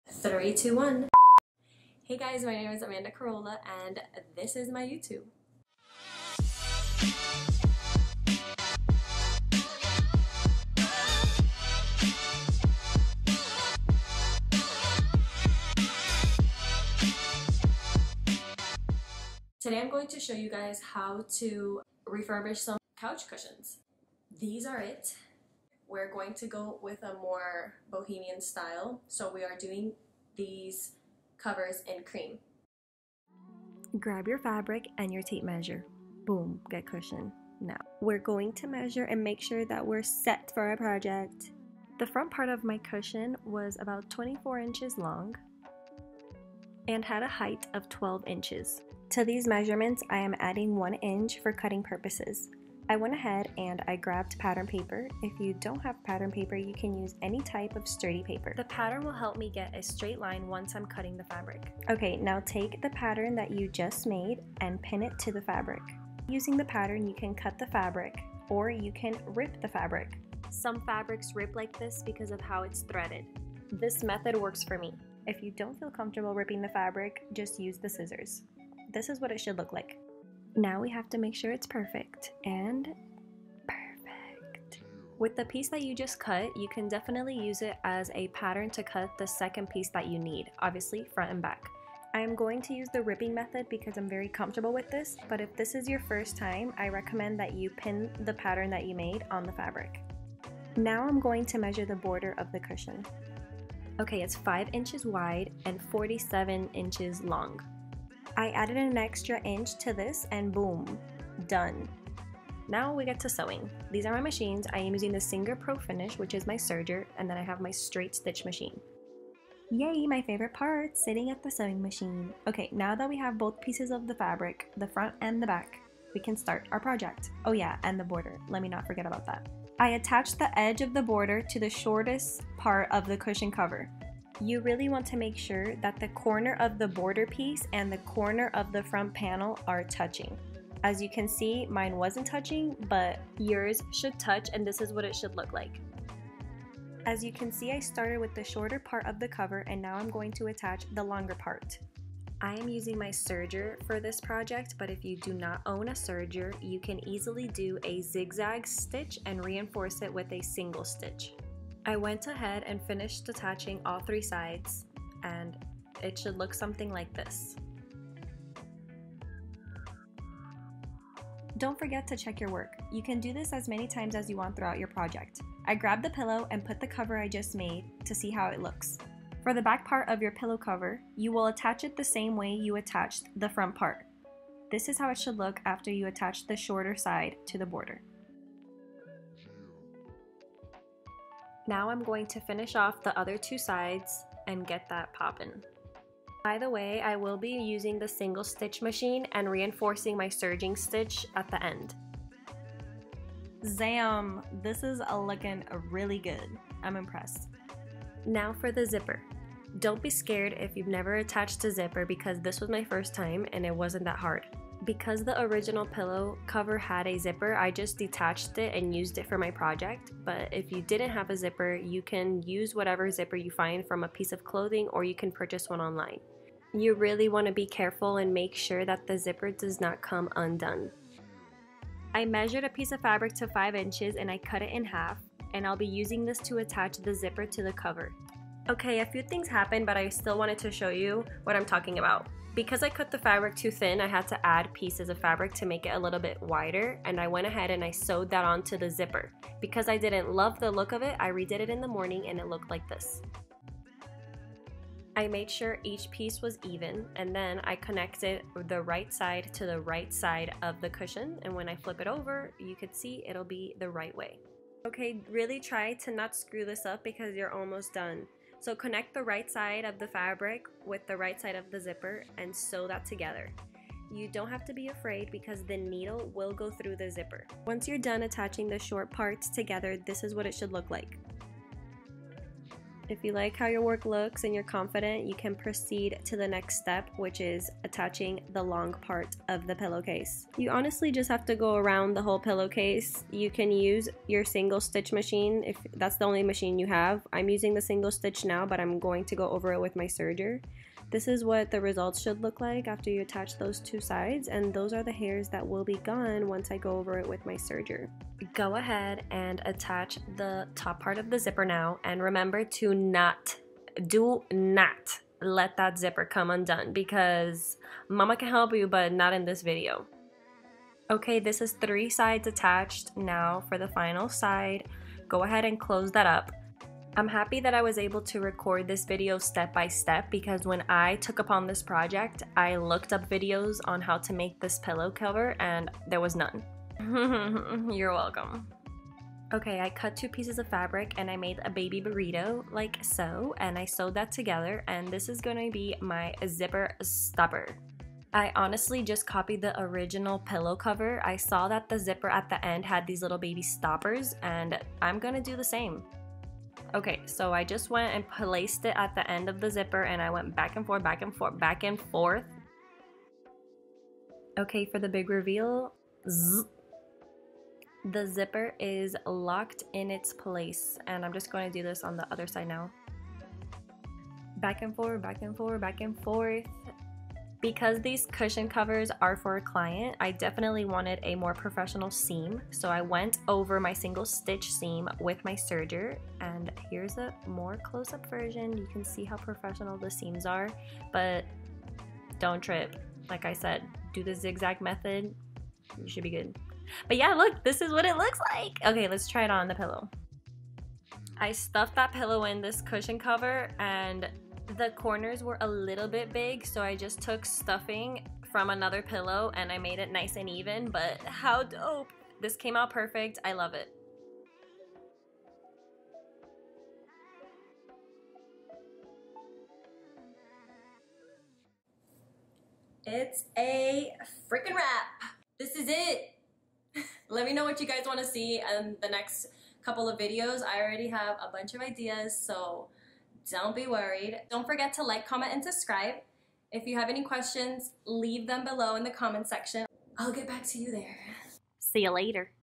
DIY: How to REFURBISH couch cushion covers
Is your couch looking kind of MEH?  This video will help you spice it up and transform blah into mwah. I hope this helps!